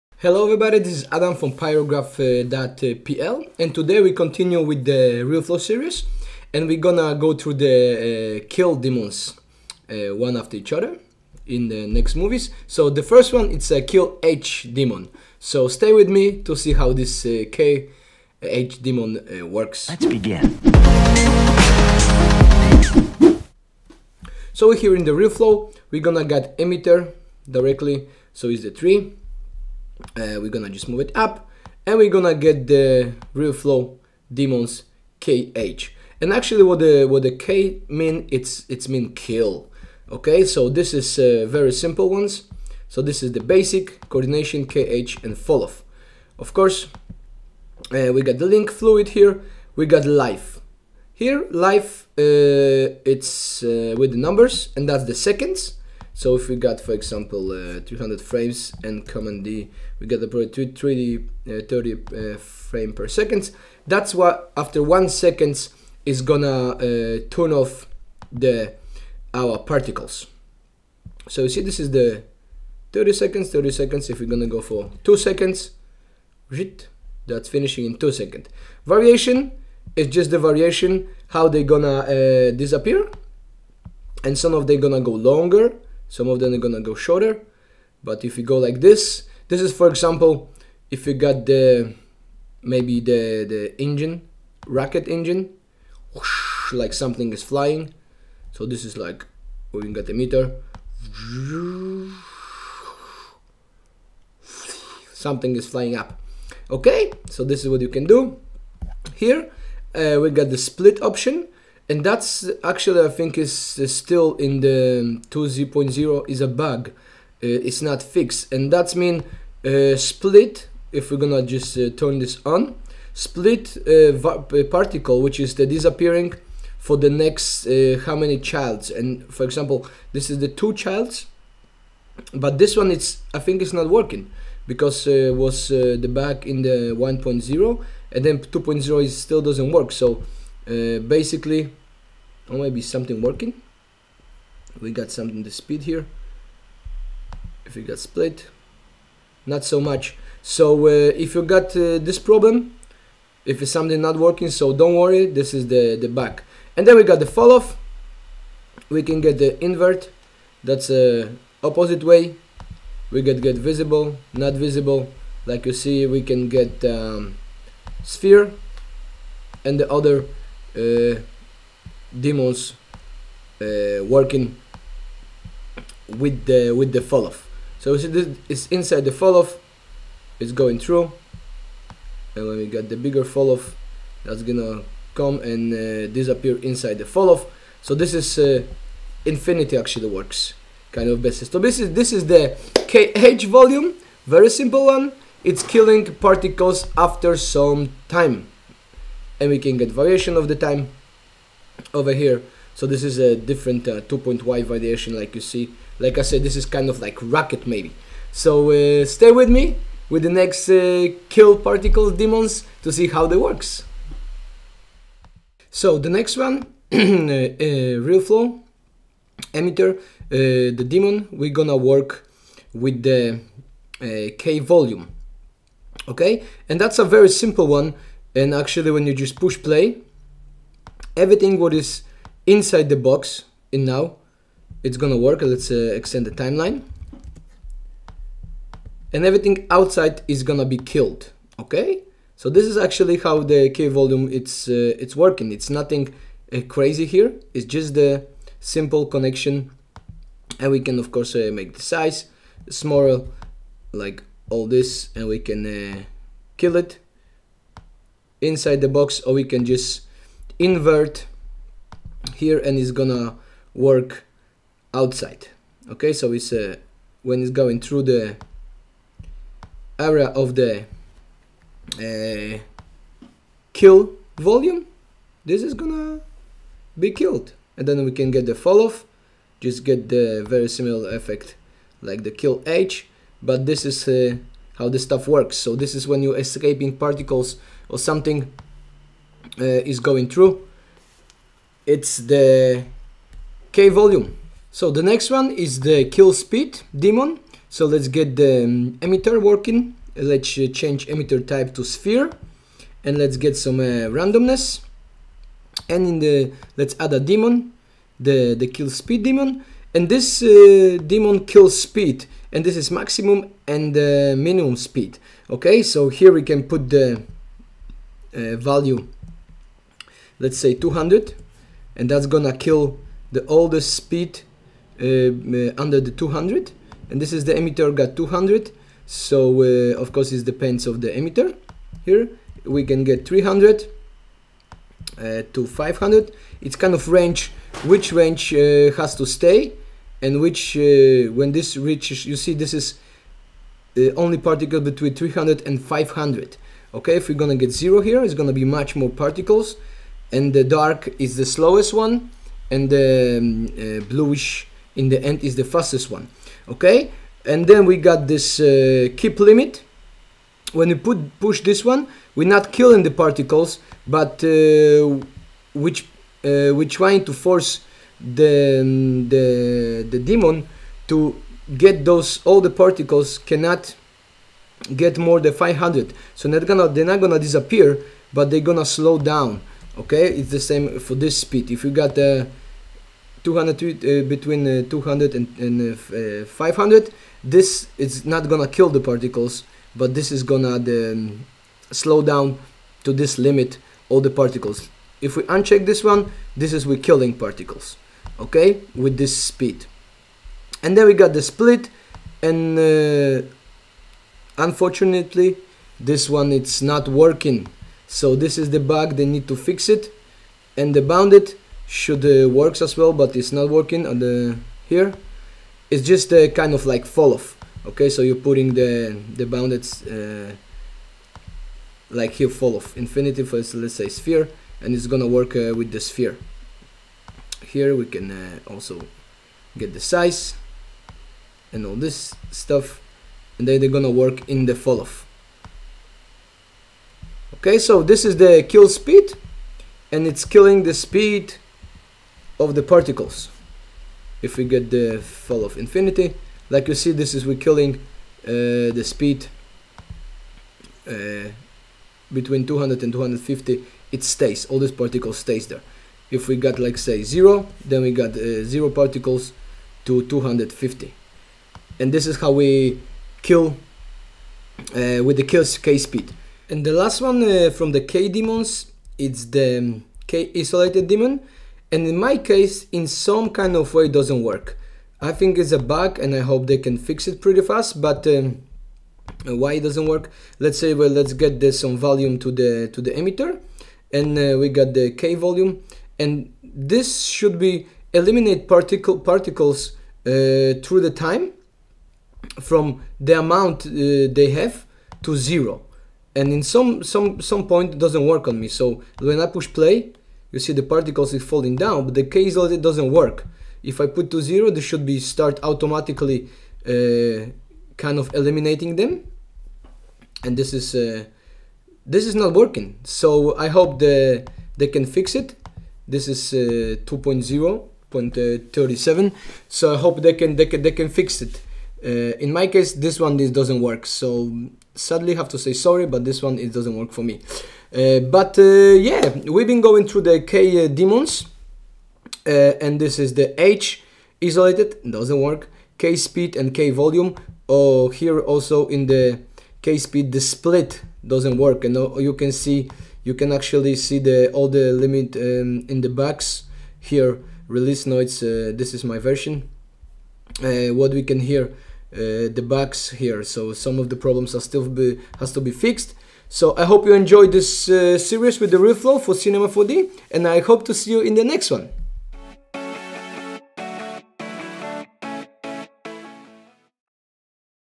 Cinema 4d Tutorial-RealFlow 2.0 Part 8 k Deamons
Explanation how Kill Deamons works in RealFlow plugin for C4d